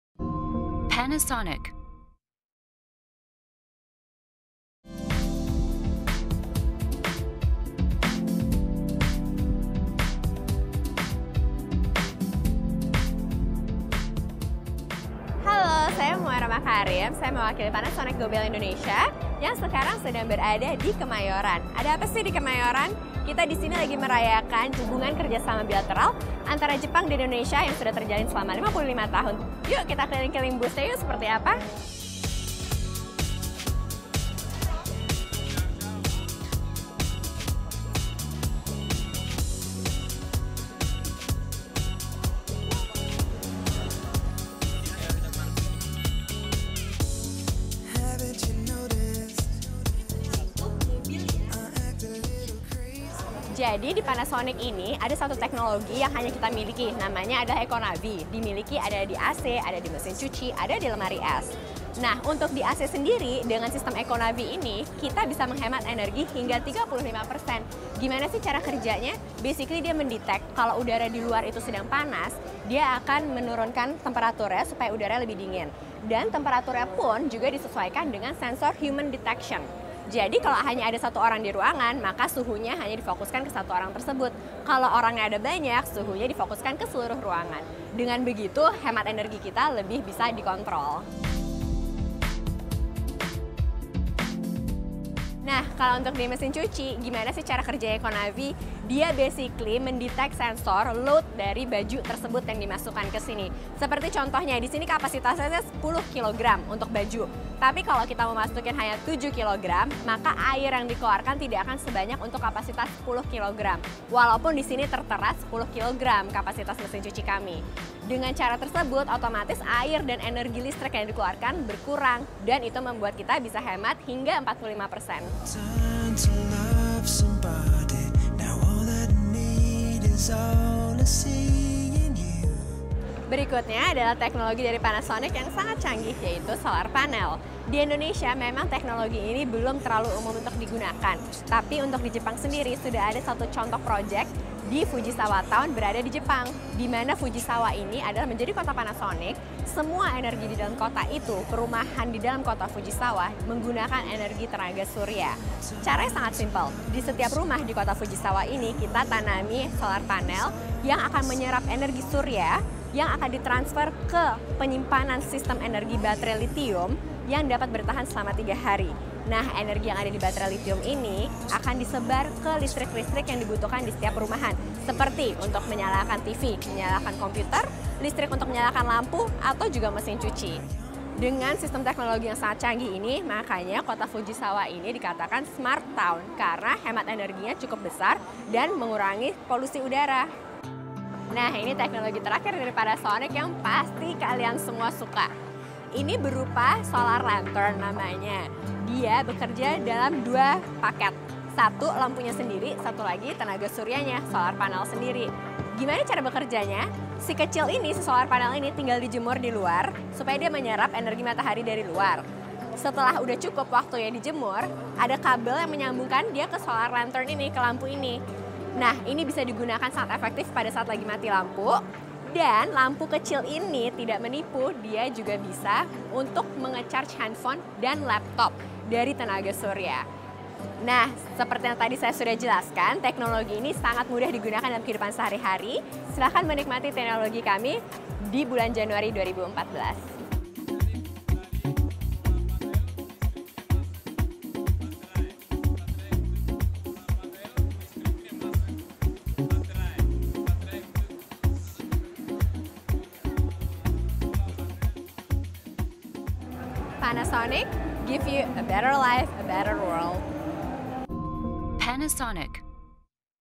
Indo - Japan Expo 2013
Panasonic Indonesia menampilkan produk dengan teknologi terkini pada pameran Indo - Japan Expo 2013 yang berlangsung akhir tahun lalu.

Konsep yang ditampilkan adalah bagaimana Panasonic mempunyai solusi pintar dan juga tentunya ramah lingkungan terhadap permasalahan di Indonesia.

Bagaimana Panasonic menyelesaikan masalah ini? Kamu bisa lihat videonya di sini.